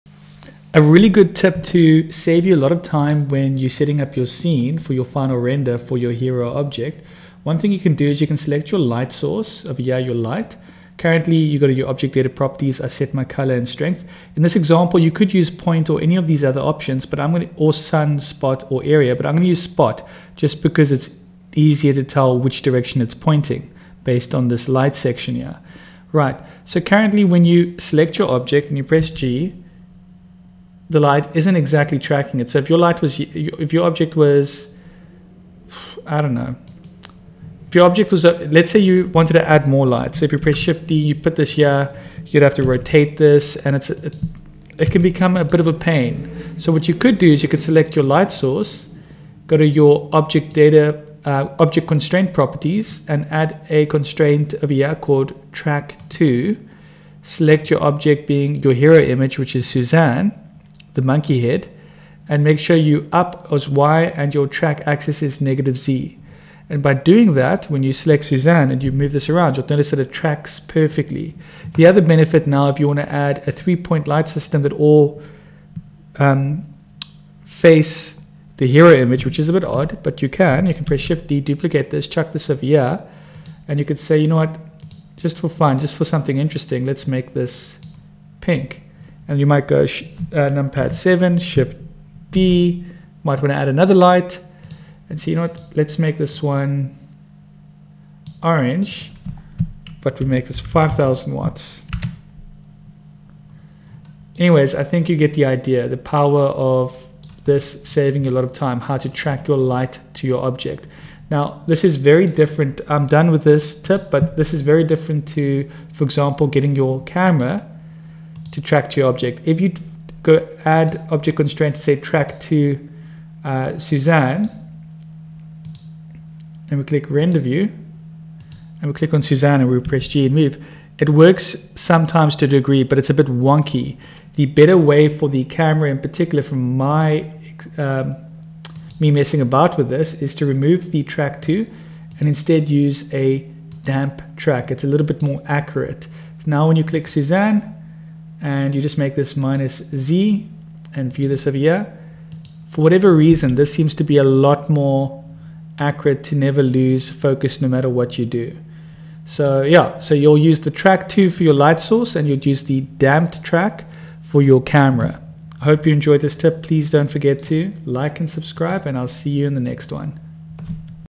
How to track light to an object in blender
In this tutorial we show you how to save time by tracking light to an object and then we compare this method to how you track a camera to an object in blender.